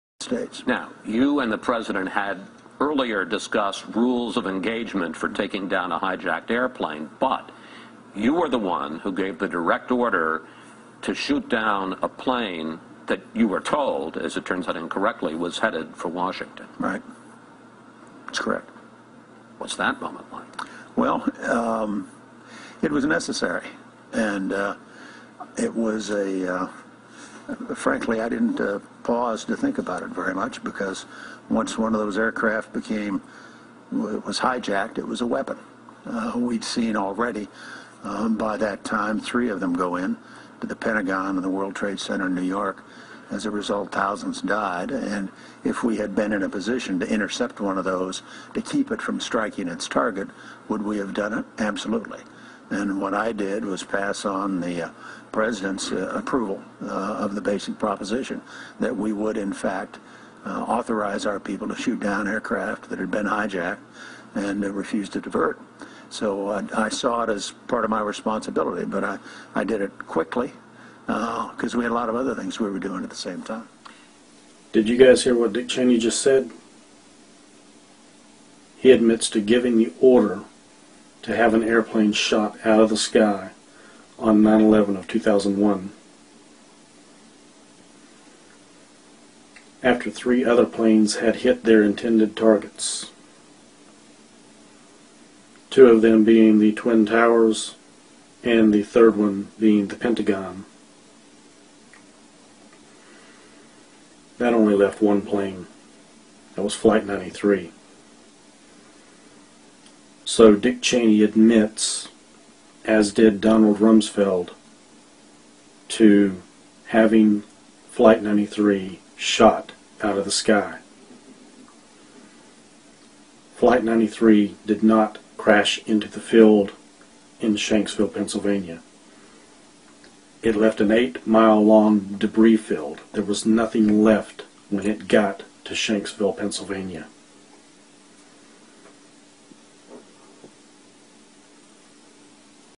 9/11 Dick Cheney admits Flight 93 was shot down
9/11   Dick Cheney admits Flight 93 was shot down